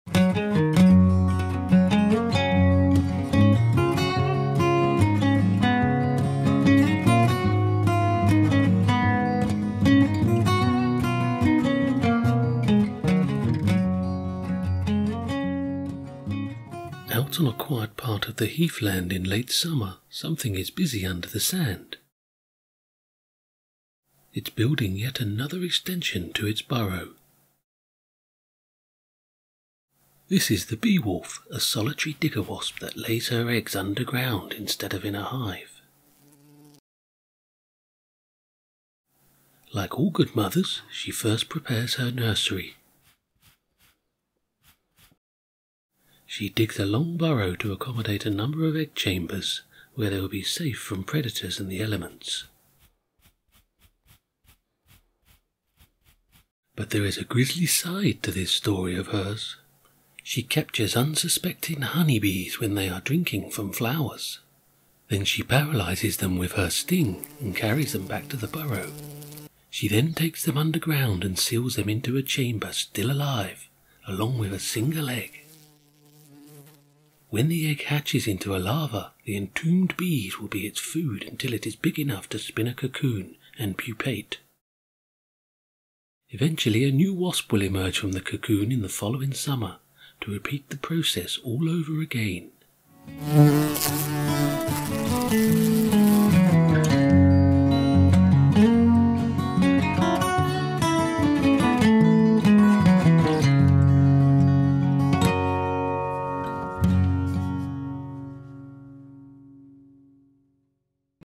Lifecycle of the European Beewolf wasp - short story with narration
A short story on the European Beewolf Wasp (Philanthus triangulum) showing how it preys on others and what it does to improve the success of its offspring. Shot on Heathland